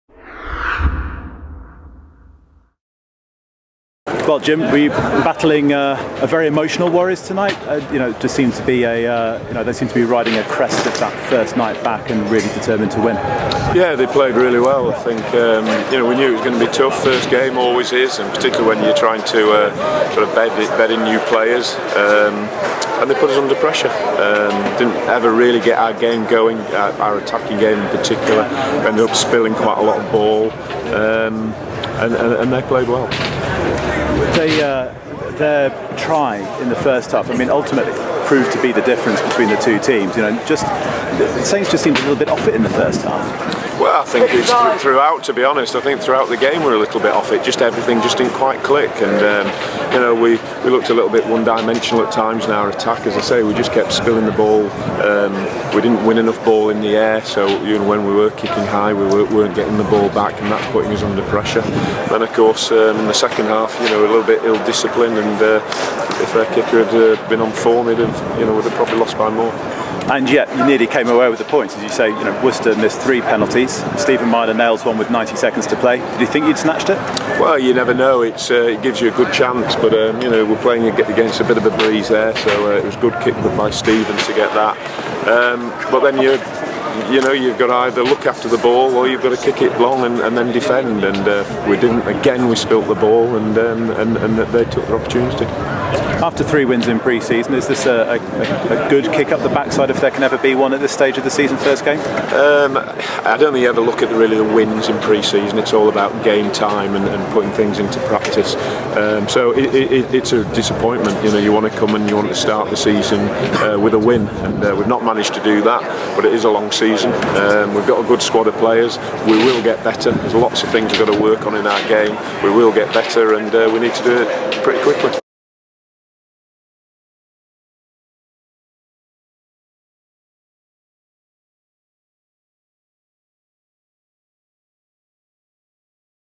WORCS 13 SAINTS 12 Mallinder reaction
Jim Mallinder says the Saints need to improve and do so quickly after an opening day defeat to Worcester in the Aviva Premiership.